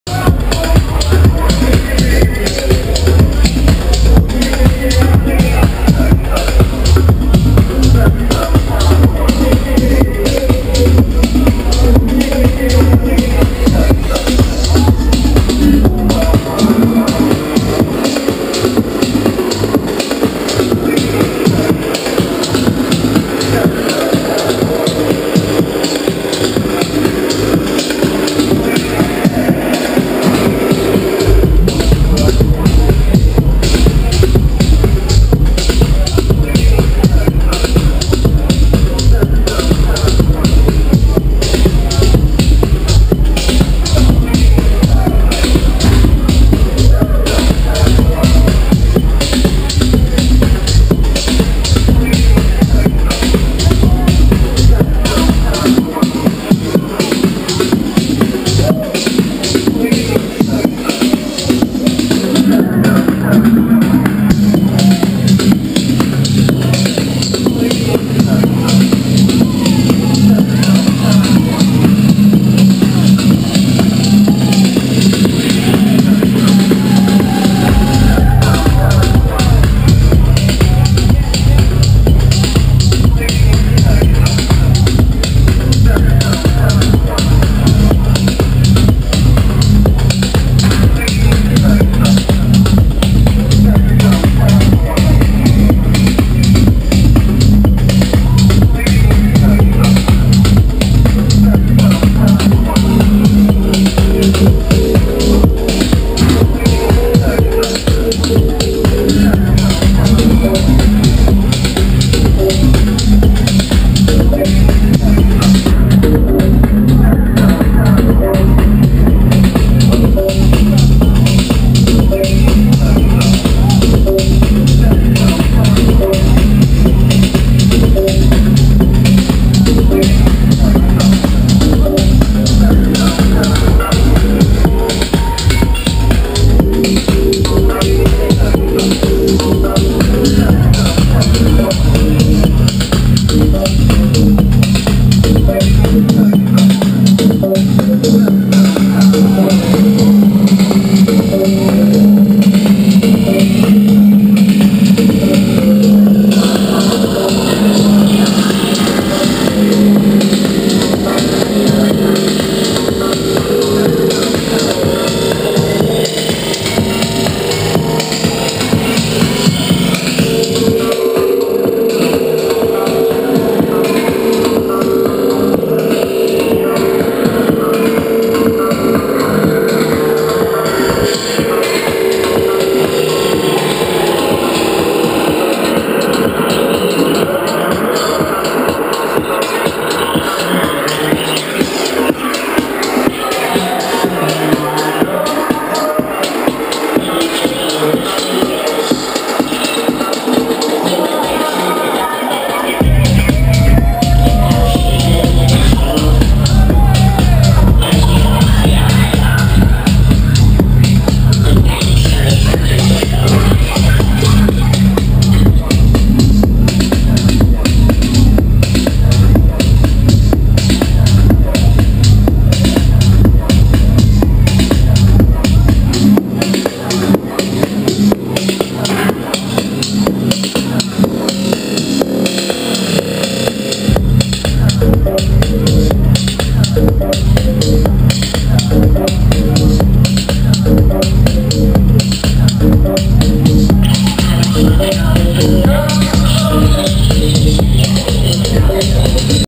MARTINEZ (Moon Harbour) playing @ SOL CITY Edition 2, Opéra Bouffe Toulouse
Video part of MARTINEZ Dj set at the last SOL CITY party @ Opera Bouffe Toulouse
Local djs: Dirty B., Dioud Poppa, Pepper & Salt